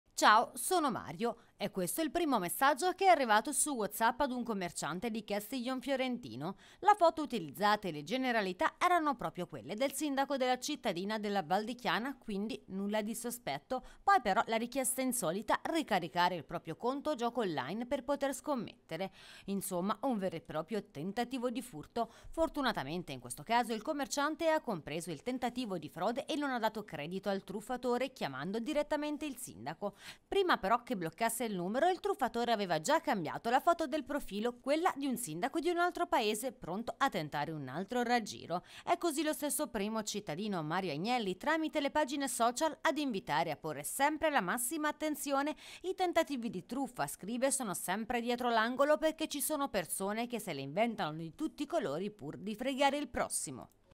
“Sono Mario, aiutami”. A Castiglion Fiorentino si finge il sindaco e tenta la truffa
“Ciao sono Mario". È questo il primo messaggio che è arrivato su Whatsapp ad un commerciante di Castiglion Fiorentino.
La foto utilizzata e le generalità erano proprio quelle del sindaco della cittadina della Valdichiana, quindi nulla di sospetto.
Poi però la richiesta insolita, ricaricare il proprio conto gioco online per poter scommettere.
Insomma un vero e proprio tentativo di furto. Fortunatamente in questo caso il commerciante ha compreso il tentativo di frode e non ha dato credito al truffatore, chiamando direttamente il sindaco.